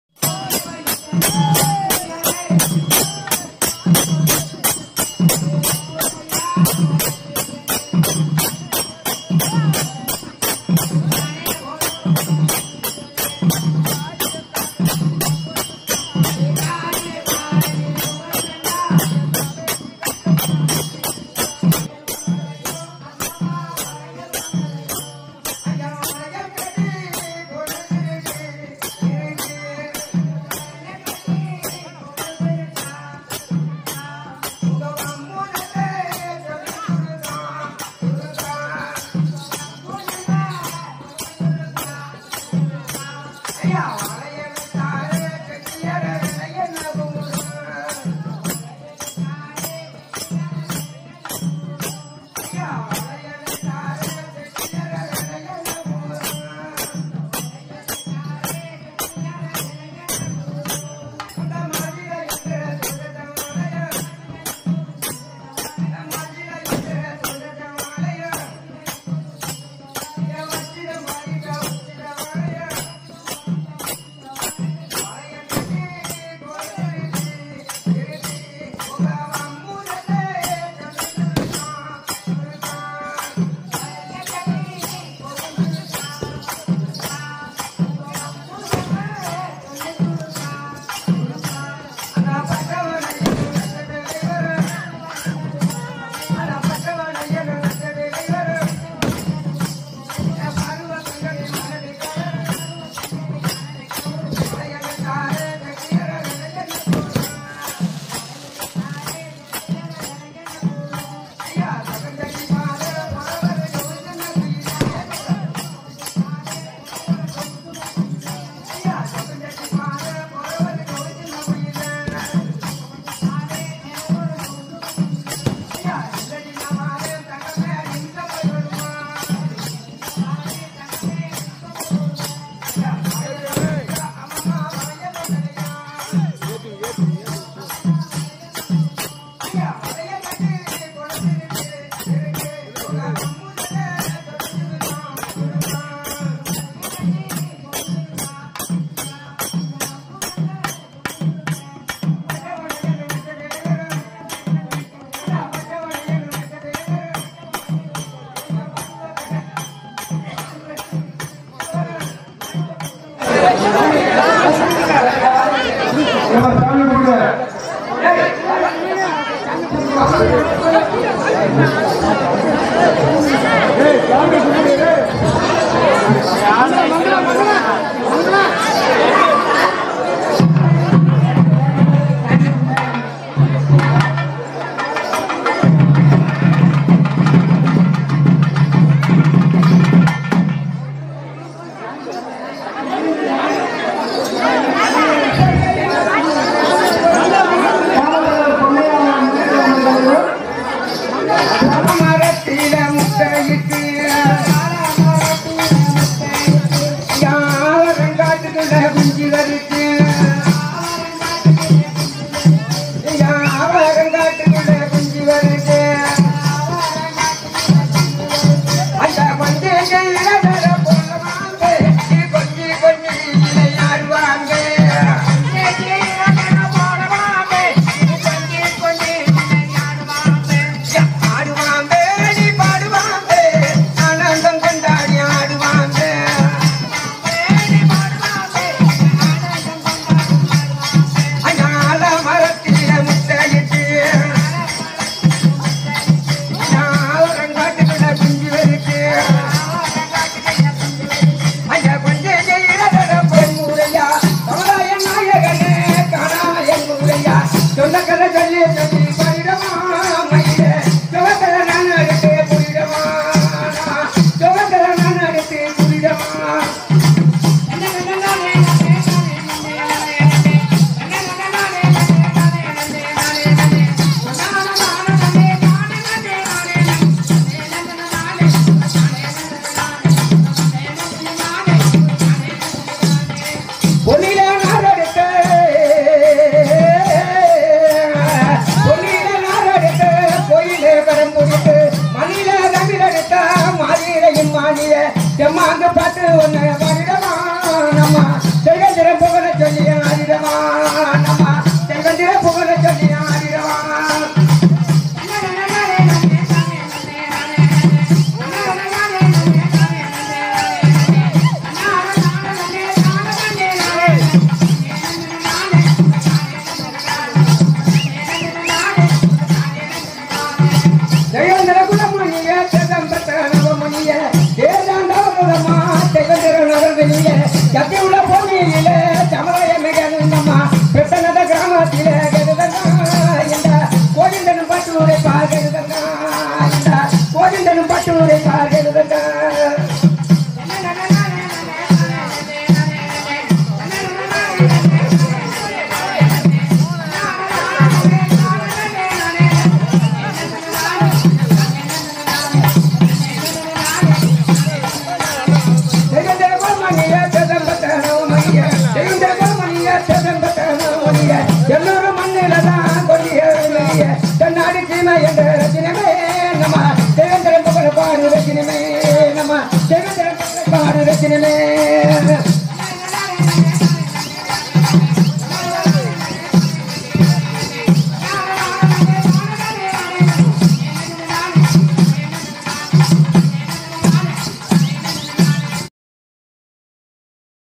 muthukulathur Kumarakuruchi mulaipaari vizha
Ramanathapuram District, Muthukulathur District, Kumarakuruchi village Sri Vaala Vantha Amman kovil Mulaipaari Thiruvizha